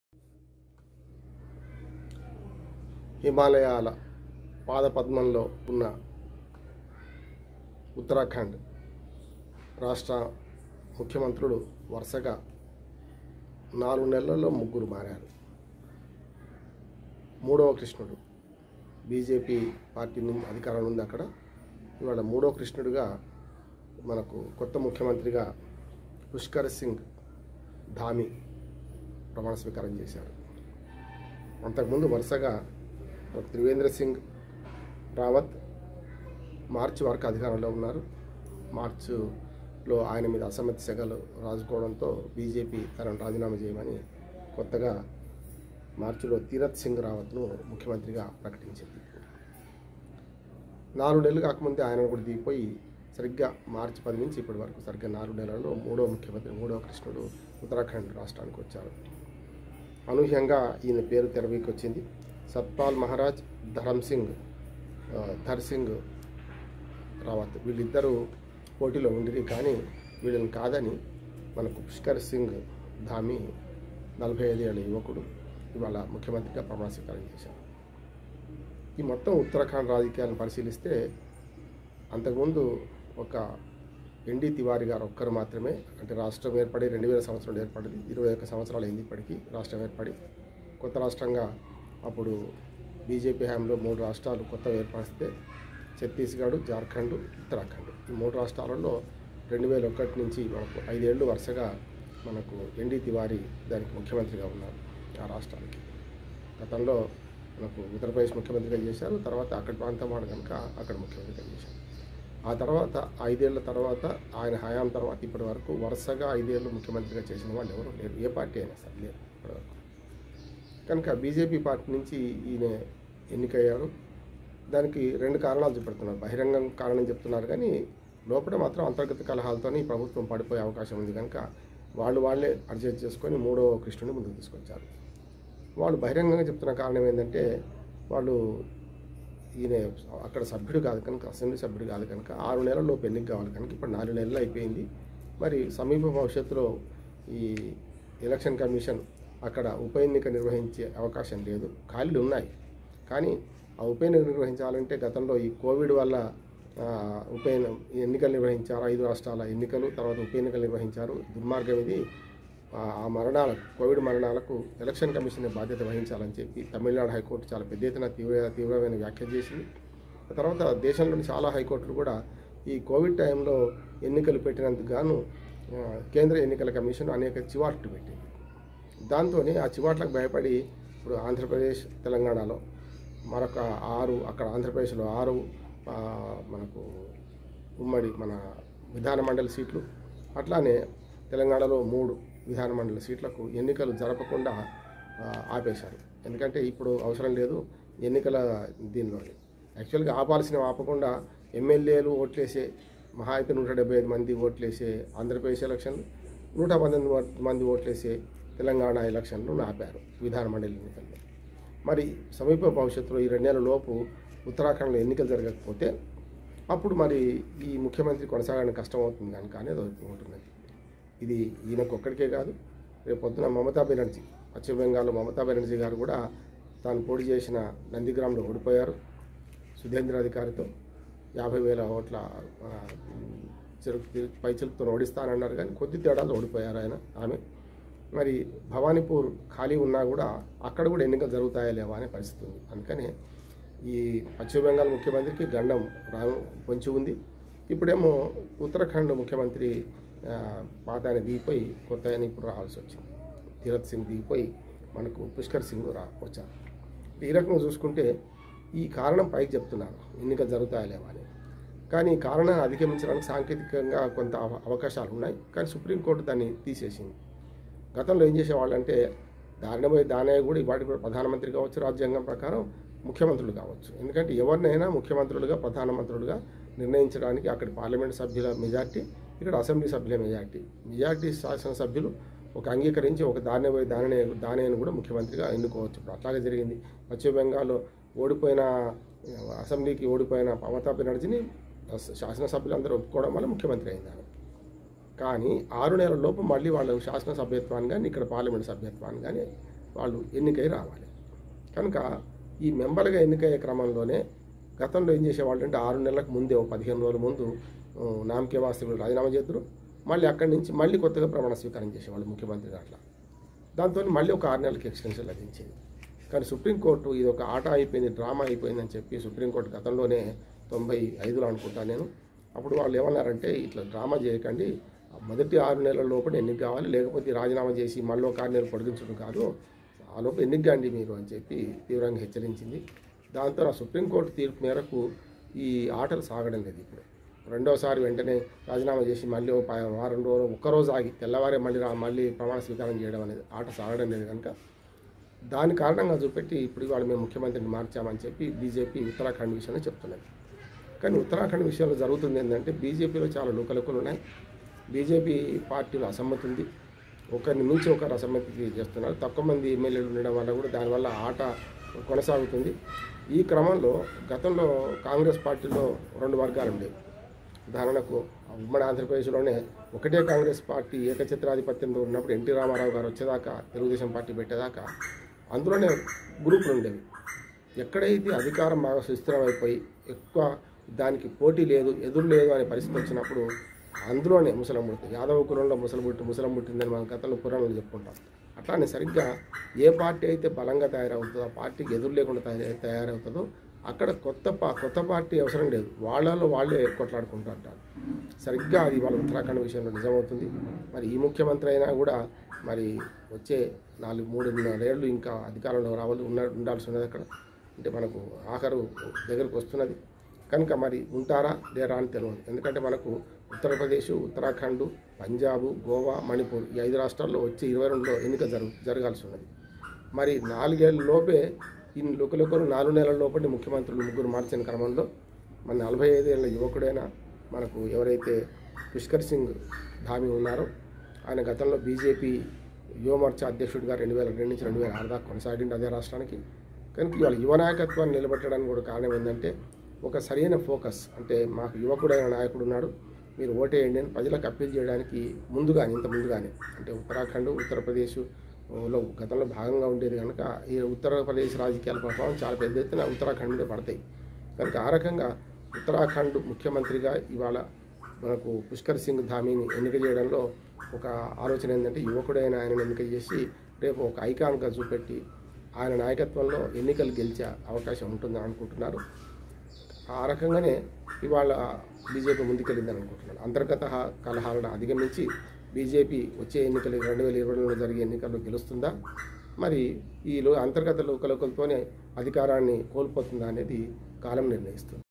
ఉత్తరాఖండ్ రాష్ట్రంలో ముచ్చటగా మూడవ కృష్ణుడు | ముఖ్యమంత్రిగా పుష్కర సింగ్ ధామీ ప్రమాణ స్వీకారం.
ఉత్తరాఖండ్ రాష్ట్రంలో ముచ్చటగా మూడవ కృష్ణుడు.

 ముఖ్యమంత్రిగా పుష్కర సింగ్ ధామీ ప్రమాణ స్వీకారం

ఉత్తరాఖండ్ బిజెపిలో లుకలుకలు ఇప్పటికైనా సమసిపోతాయా?.